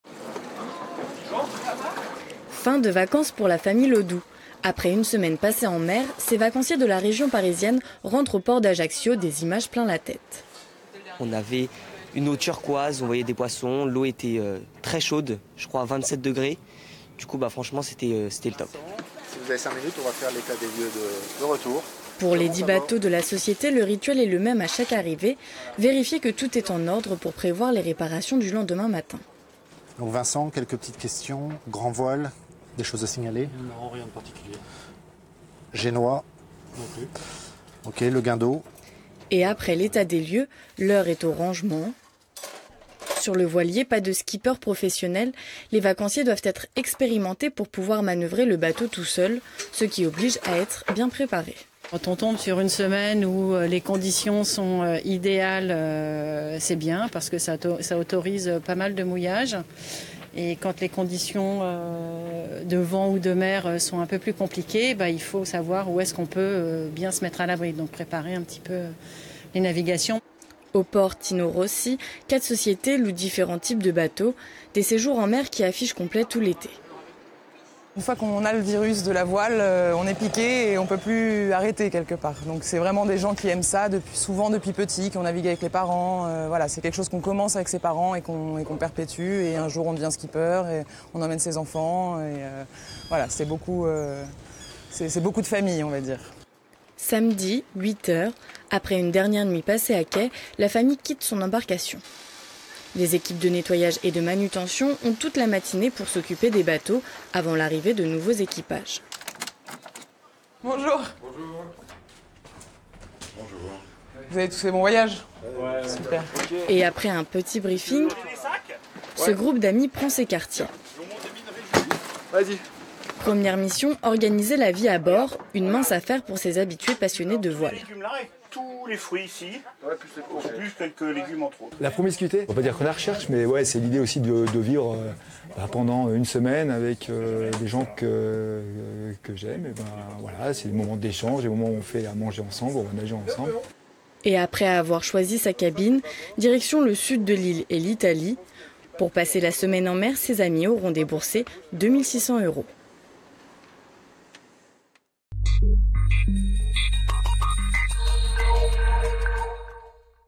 Découvrir la Corse en bateau
Découvrir la Corse depuis la mer...
C'est ce que choisissent de plus en plus de vacanciers.
En juillet et août, les sociétés insulaires de locations de voiliers affichent complets.
Pour une semaine à bord à 4 ou 5, il faut compter autour de 2500 euros.

Intervenants -
Axel Laudou
Vacancier
Sandra Laudou
Vacancière
Edmée Plaisant
Présidente de la société Corsazur
Pierre Morize
Vacancier

Equipe -
PERROUX Ophélie
BONTE BARATCIART Cécile
SEBAYHI Alexandra